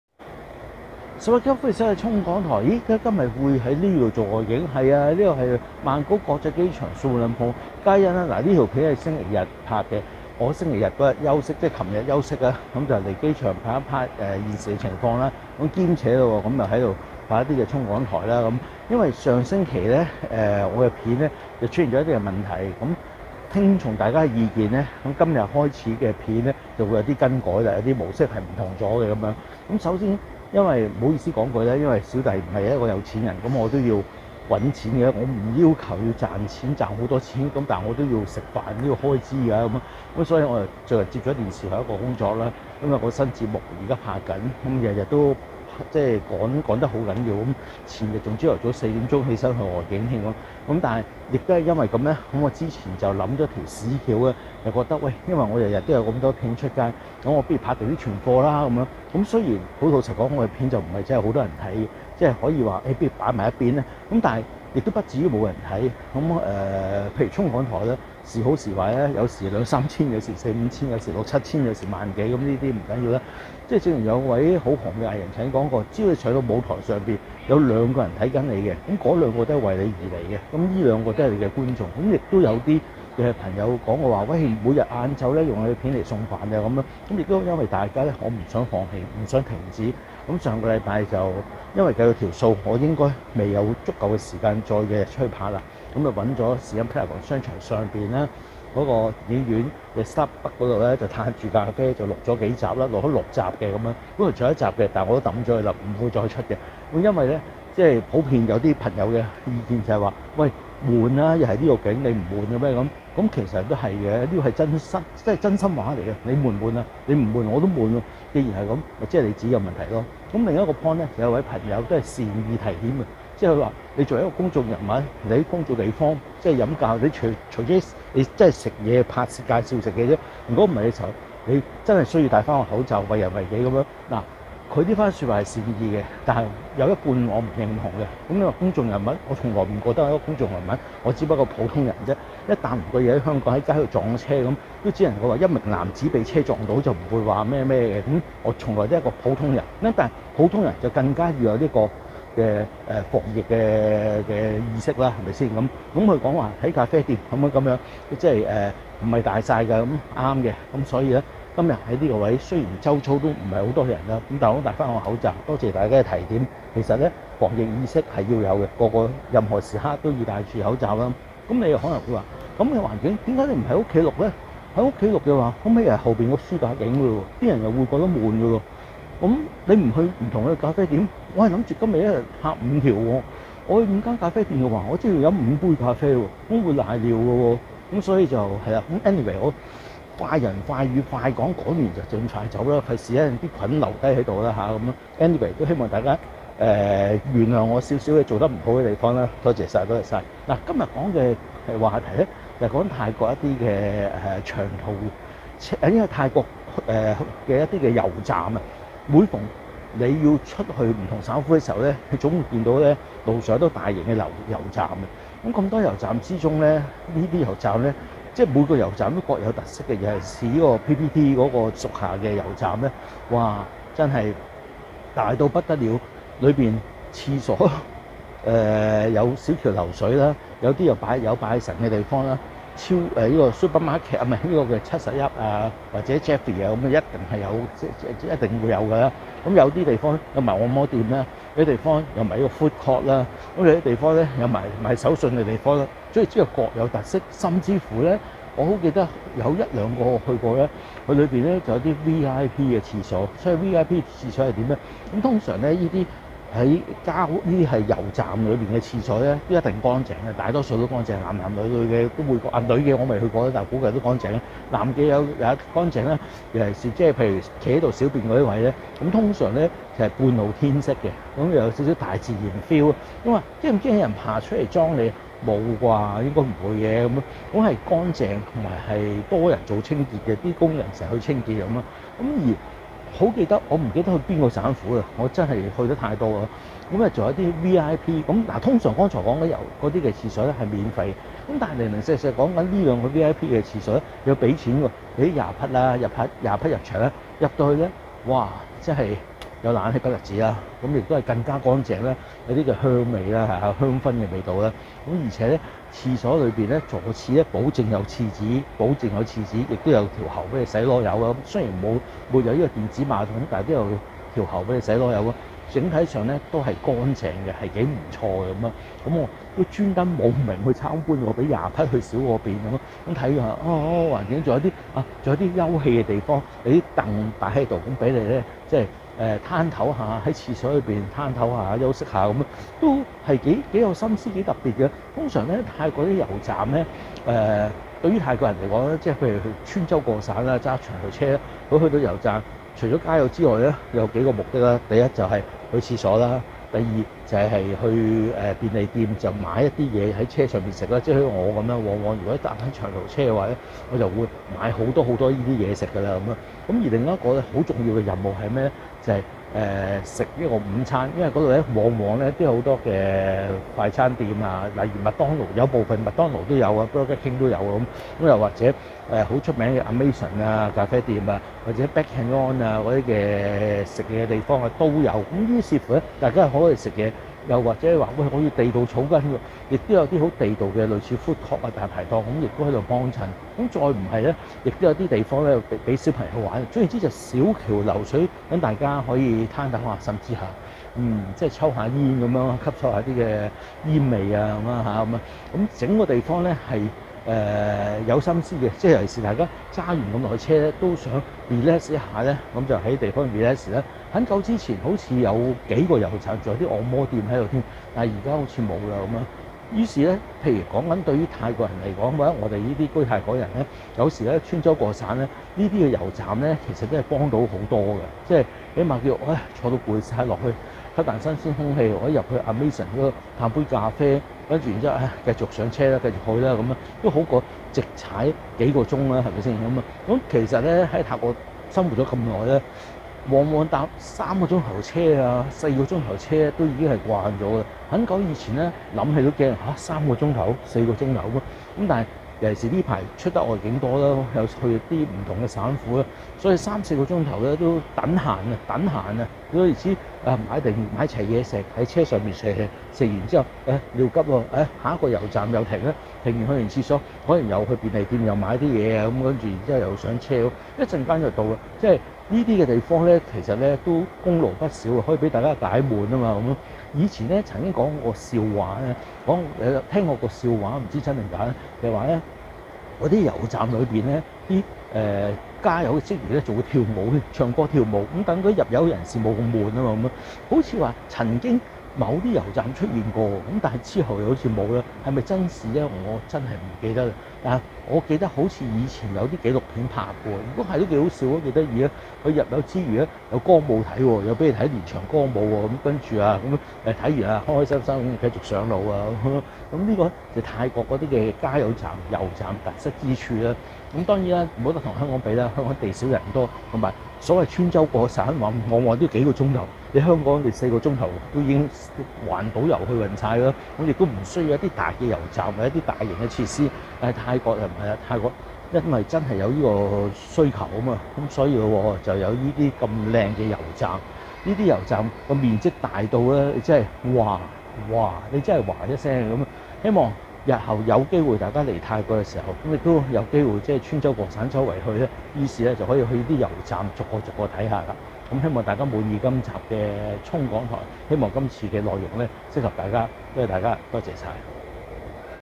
【冲港台】💖 泰國長途車入油站有何特色？超大地方、有Cafe、按摩店、靚厠所，職員還會表演跳舞？~✹香港#移居泰國 旅遊達人Roger Wu胡慧冲 主持 ►Roger's Gossip
🐦 近期拍攝電視節目經常穿州過省，最常接觸就是泰國的長途車入油站。 究竟泰國長途車入油站有何特色？... 超大地方、有Cafe、按摩店、靚厠所，職員還會表演跳舞？~✹香港移居泰國 旅遊達人Roger Wu胡慧冲 主持  ►Roger's Gossip (2022/4/4)  【冲港台】